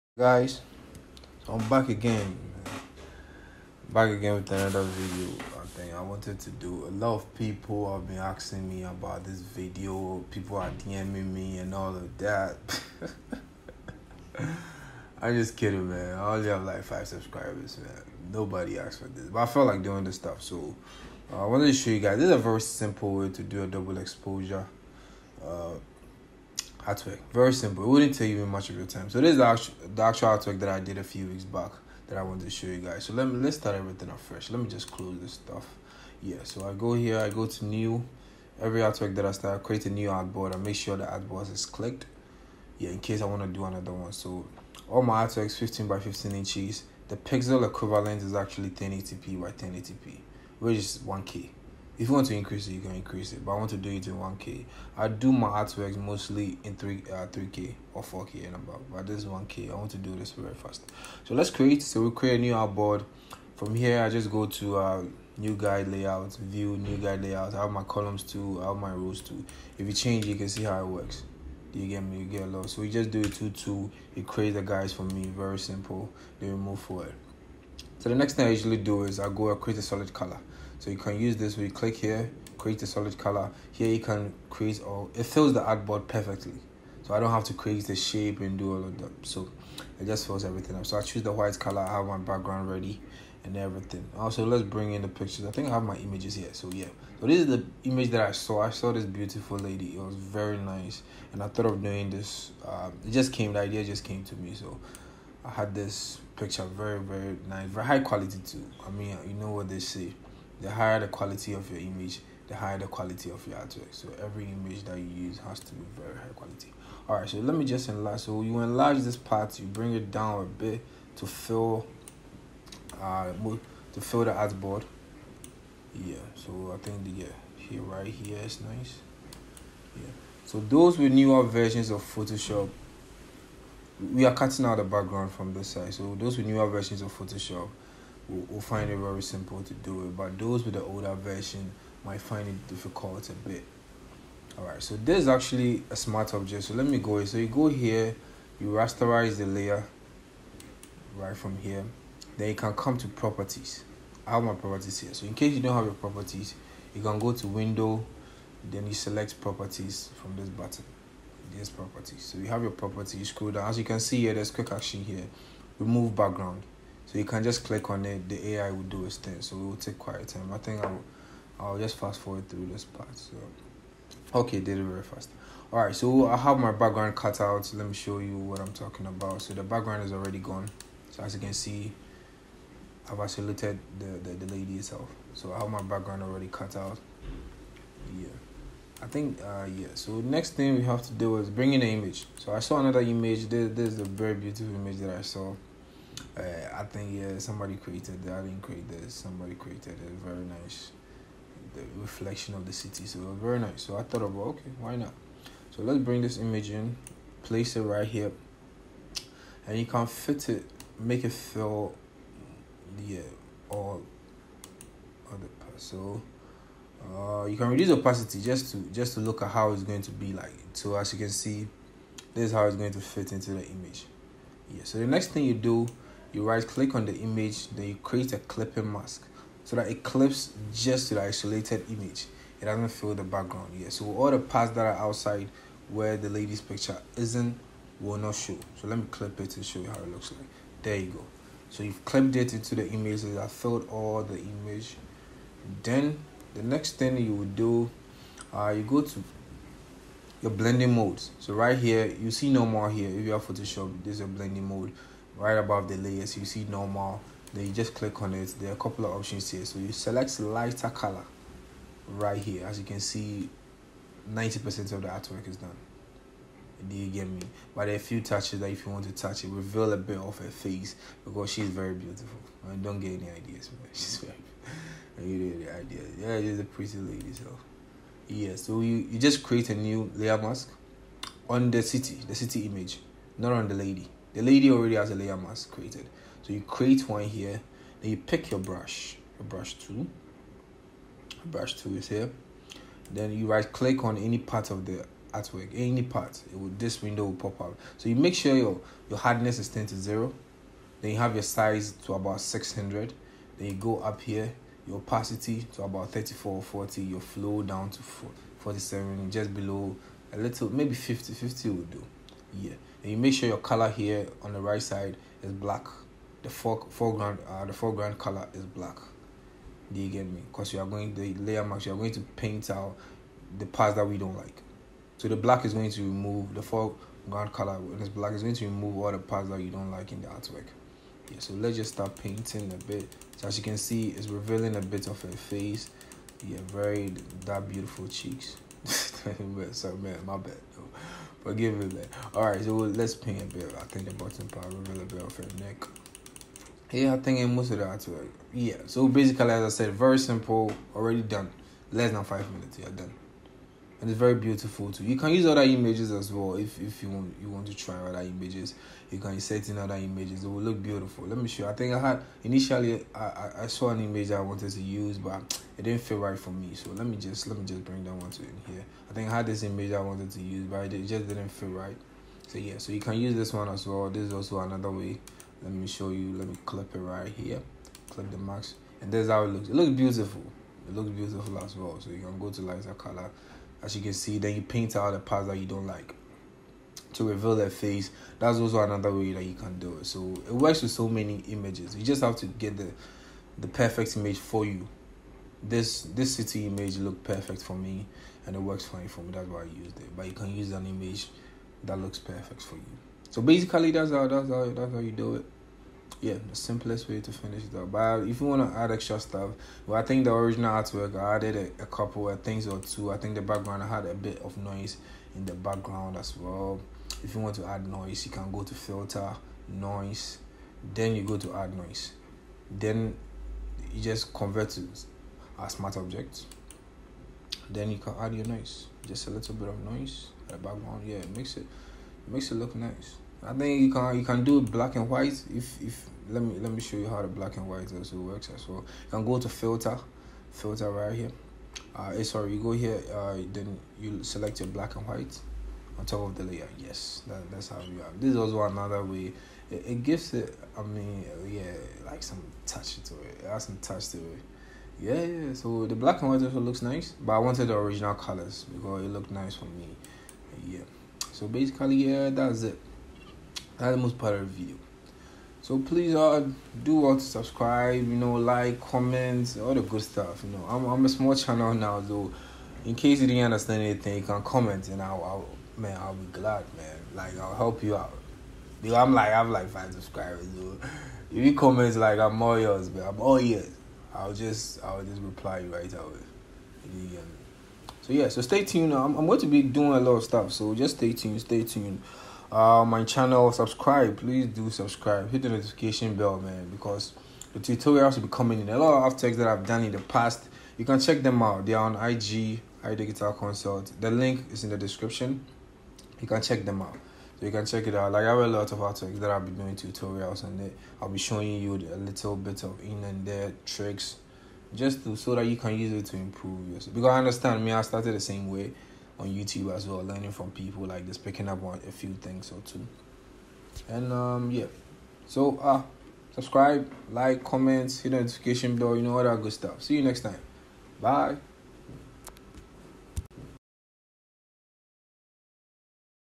Create a Double Exposure in 5min with Photoshop
The Easiest Way to Create a Double Exposure Effect in Photoshop! In this tutorial, learn how to use the basics of Masking and Blend Modes to create a simple but elegant double exposure image.